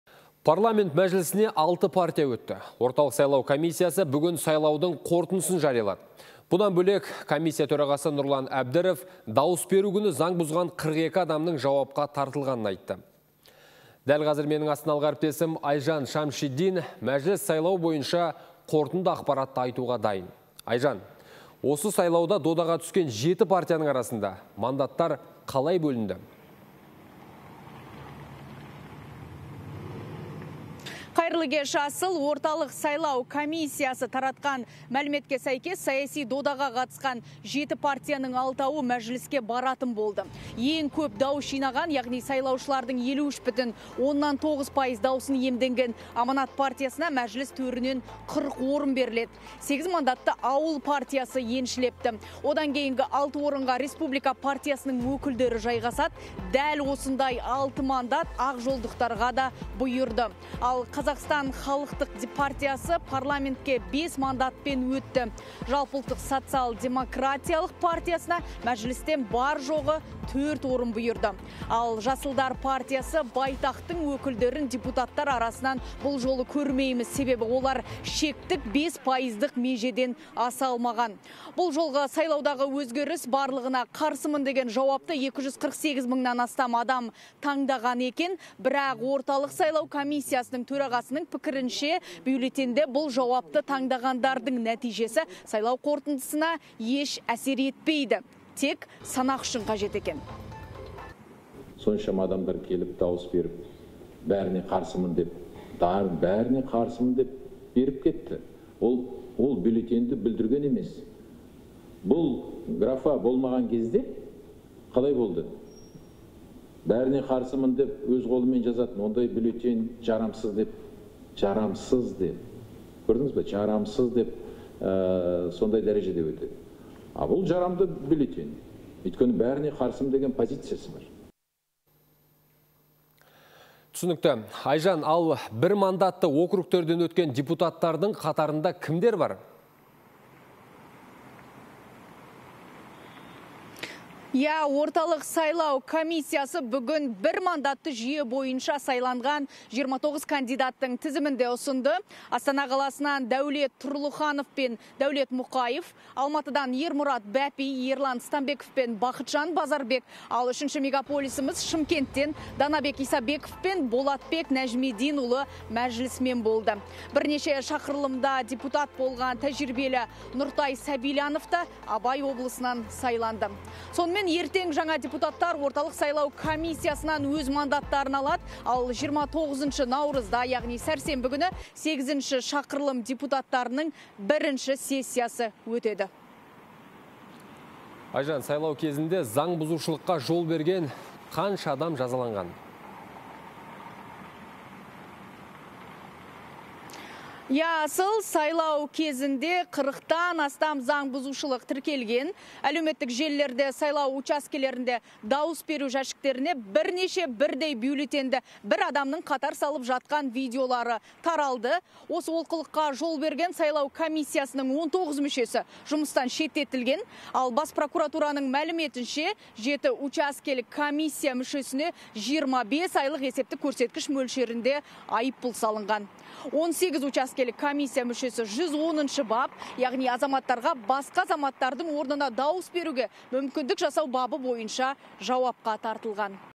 Сайлау-2023: Парламент Мәжілісіне алты партия өтті
Парламент Мәжілісіне алты партия өтті. Орталық сайлау комиссиясы бүгін сайлаудың қорытындысын жариялады.  Бұдан бөлек, комиссия төрағасы Нұрлан Әбдіров дауыс беру күні заң бұзған 42 адамның жауапқа тартылғанын айтты. Дәл қазір менің астаналық әріптесім Айжан Шамшидин мәжіліс сайлауы бойынша қорытынды ақпаратты айтуға дайын.
 Орталық сайлау комиссиясы таратқан мәліметке сәйкес, саяси додаға қатысқан жеті партияның алтауы мәжіліске баратын болды. Ең көп дауыс жинаған, яғни сайлаушылардың 53,9 % даусын иемденген "Аманат" партиясына Мәжіліс төрінен 40 беріледі. 8 мандатты Ауыл партиясы еншіледі. Одан кейінгі алты орынға Республика партиясының өкілдері жайғасады. Дәл осындай алты мандат Ақжолдықтарға да бұйырды. Ал Қазақстан халықтық партиясына Парламентке 5 мандатпен өтті. Жалпыұлттық социал-демократиялық партиясына Мәжілістен бар-жоғы төрт орын бұйырды. Ал  Жасылдар партиясы "Байтақтың" өкілдерін депутаттар арасынан көрмейміз. Себебі олар шектік 5 пайыздық межеден аса алмаған. Бұл жолғы сайлаудағы өзгеріс «Барлығына қарсымын» деген жауапты 248 мыңнан астам адам таңдаған екен. Бірақ Орталық сайлау комиссиясы төрағасының пікірінше бюллетеньде бұл жауапты таңдағандардың нәтижесі сайлау қорытындысына әсер етпейді. Тек санақ үшін қажет.
НҰРЛАН ӘБДІРОВ, ҚР Орталық сайлау комиссиясының төрағасы:
– Соншама адамдар келіп, барлығына қарсымын деген дауысты беріп кетті. Ол бюллетеньді бүлдірген емес. Бұл графа болмаған кезде қалай болды? Мен барлығына қарсымын деп қолдан жазатын. Ондай бюллетень жарамсыз деп танылатын. Ал бұл жарамды бюллетень. Өйткені барлығына қарсымын деген позициясы бар.
Ал, бір мандатты округтерден өткен депуттардың қатарында кімдер бар?
Орталық сайлау комиссиясы бүгін бір мандатты жүйе бойынша сайланған 29 кандидаттың тізімін де ұсынды. Астана қаласынан Дәулет Тұрлыханов пен Дәулет Мұқаев, Алматыдан Ермұрат Бәпи, Ерлан Стамбеков пен Бақытжан Базарбек, ал үшінші мегополис Шымкенттен Данабек Исабеков пен Болатбек Нәжметдинұлы мәжілісмен болды. Бірнеше шақырылымда депутат болған тәжірибелі Нұртай Сабильянов та Абай облысынан сайланды. Сонымен, ертең жаңа депутаттар Орталық сайлау комиссиясынан өз мандаттарын алады. Ал 29 наурызда сәрсенбі күні Сегізінші шақырым депуттарының бірінші сессиясы өтеді.
Сайлау кезінде қырықтан астам заң бұзушылық тіркелген. Әлеуметтік желілерде сайлау учаскелерінде дауыс беру жәшіктеріне бірнеше бірдей бюллетеньді бір адамның қатар салып жатқан видеолары таралды. Осы олқылыққа жол берген сайлау комиссиясының 19 мүшесі жұмыстан шеттетілген. Бас прокуратураның мәліметінше,  жеті учаскелік комиссия мүшесіне жиырма бес айлық есептiк көрсеткiш мөлшерiнде айыппұл салынған. 18 учаскелік комиссия мүшесі 110-бап, яғни азаматтарға басқа адамдардың орнына дауыс беруге мүмкіндік жасау бабы бойынша жауапқа тартылған.